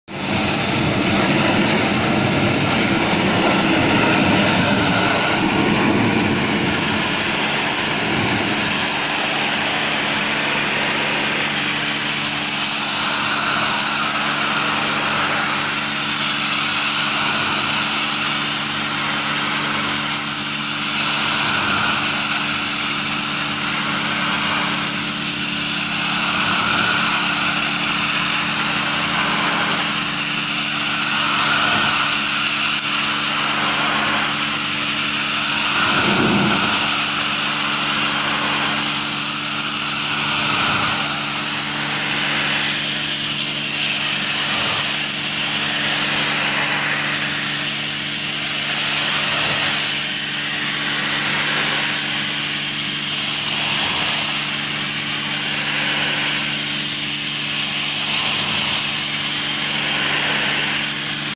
Hidrolavado frente Makro Ituzaingo con hidrolavadora alta presion.
Se hidrolavo  frente de chapa y mamposteria para sacar suciedades y pintura vieja antes de aplicar la nueva.